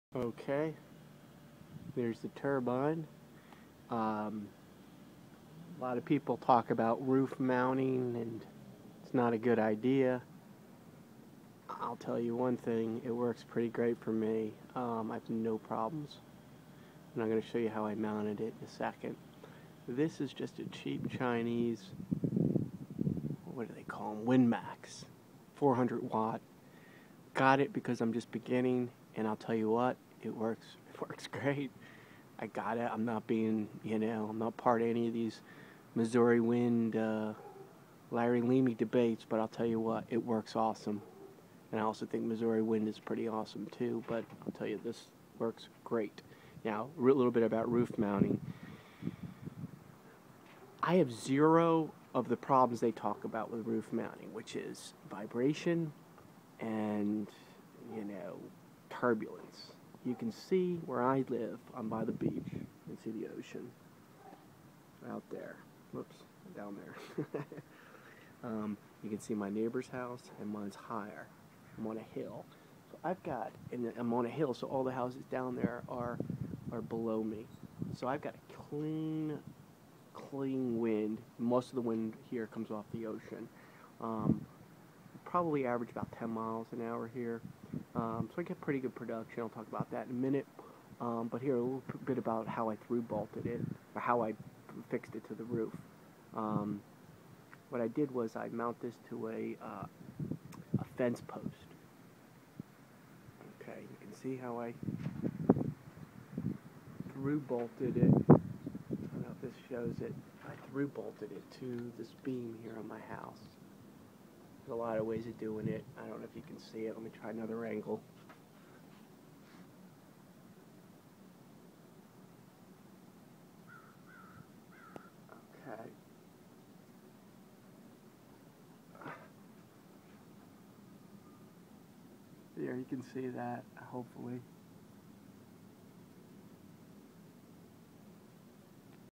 Turbine Roof Mount
A 400w Windmaz turbine mounted to my roof by through bolting the fence post it's clamped on to my beam. Just purchased a fence post from home depot and drilled 3 holes thru it. Then I bolted it down thru the beam and added 6 more bolts thru the pipe clamps. I also run post thru roof eve so snug and wrapped padding around portion going thru roof with padding to eliminate vibration.